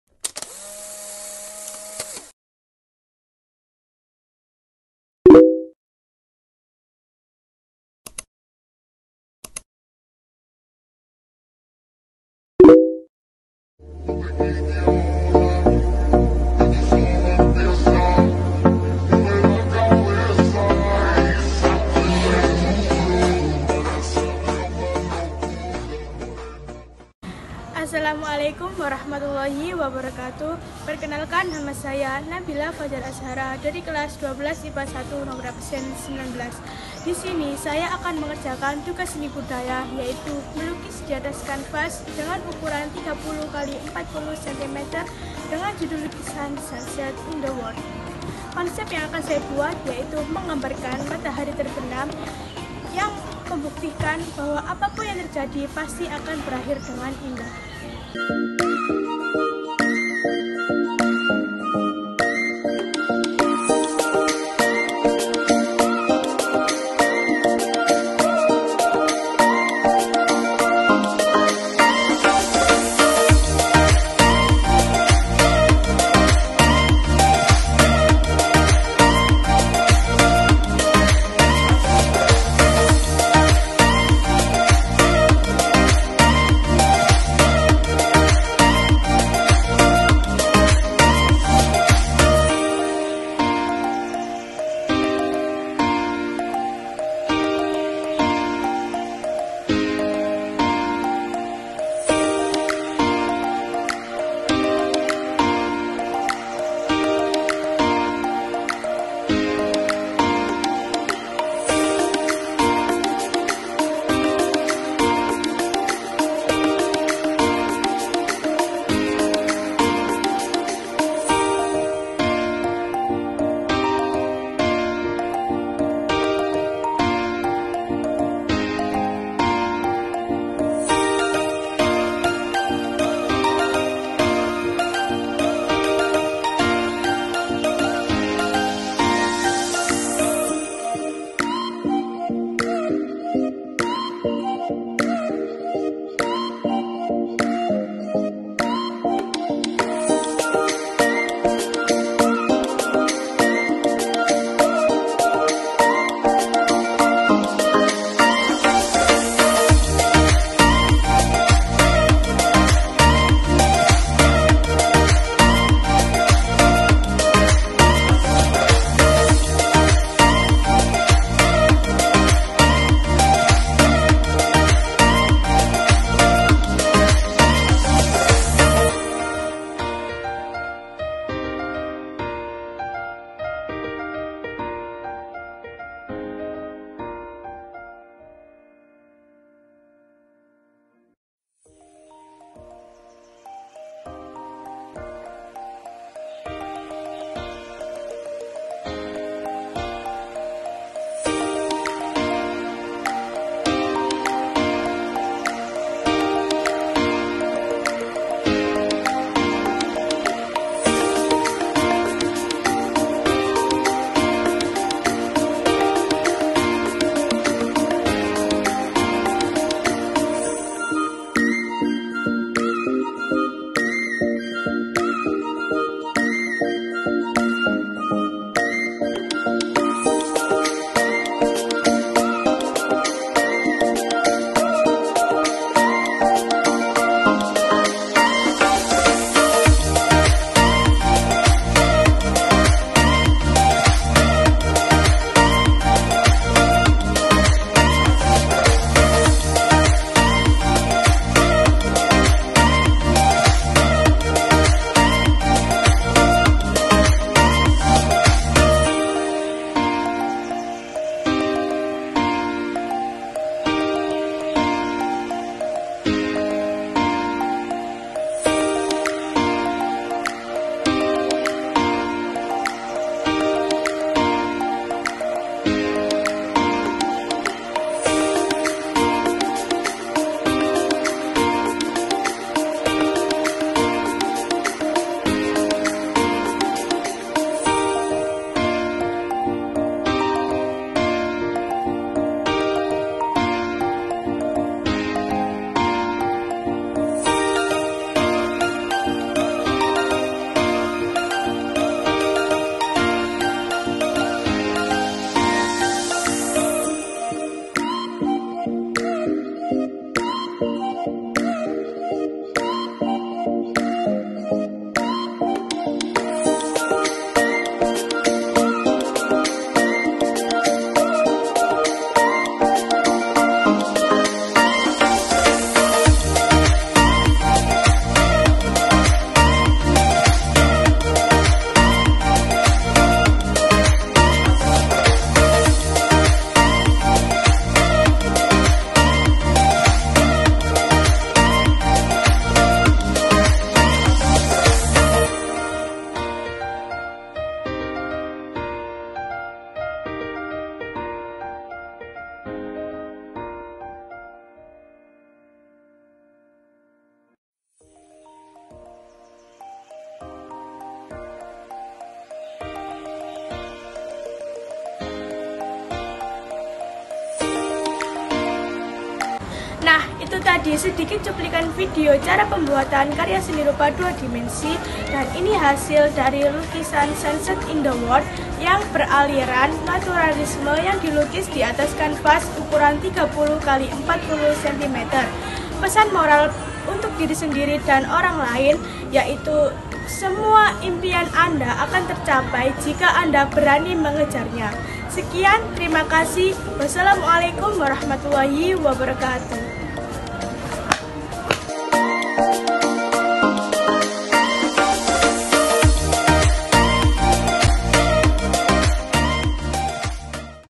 Sunset In The World_Berkarya Seni Rupa 2 Dimensi_Nabila Fajar Azhara
Nama : Nabila Fajar Azhara (19)
Judul lukisan: "Sunset In The World"
Aliran: Naturalisme Karya 2 Dimensi Yang Di Lukis Di Atas Kanvas Uk. 30x40 cm
Alat dan bahan: Kanvas,Cat Akrilik,Kuas,Palet,Pensil,Penghapus
Konsep: Menggambarkan matahari terbenam di gurun pasir yang membuktikan bahwa apa pun yang terjadi setiap hari dapat berakhir dengan indah